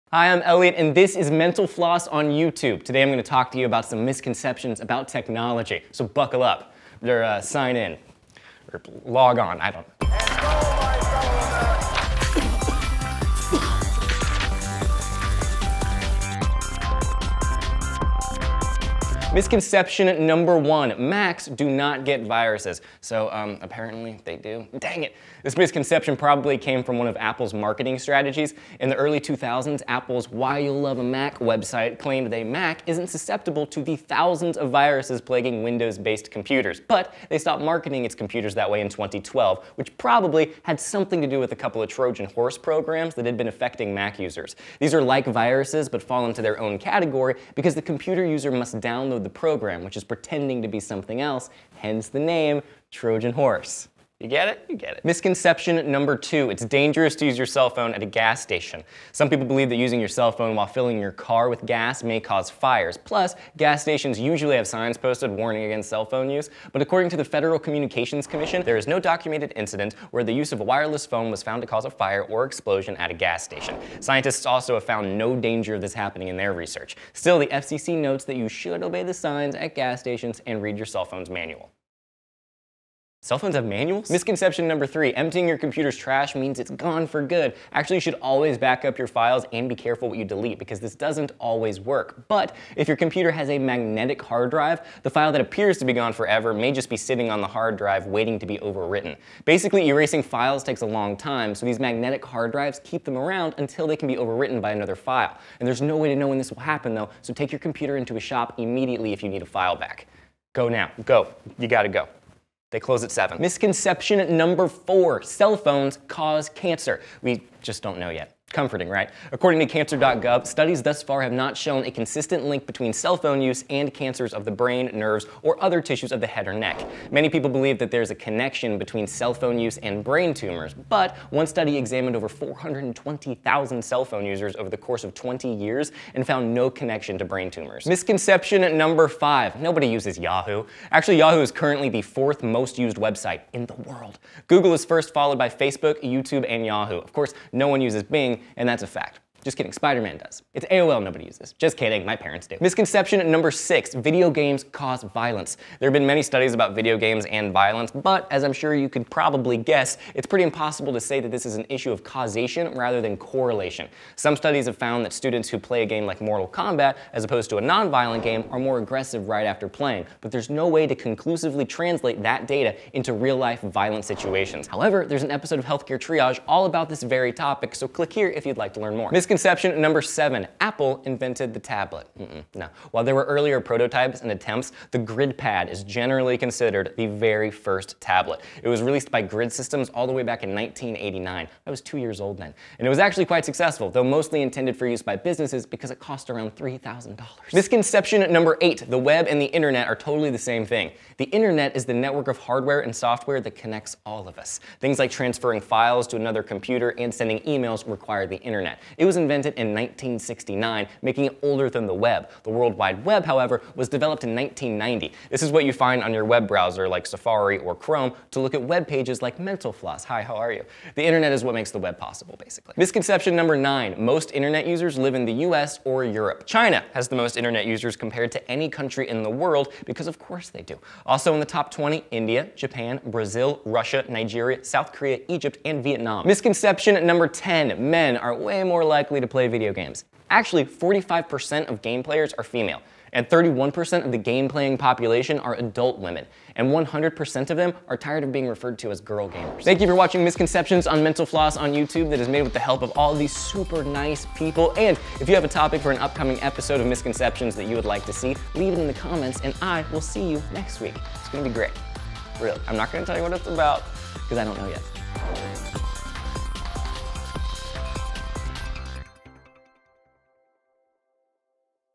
Misconceptions about Technology - mental_floss on YouTube (Ep.5)
A weekly show where we debunk common misconceptions. This week, Elliott discusses some misconceptions about technology.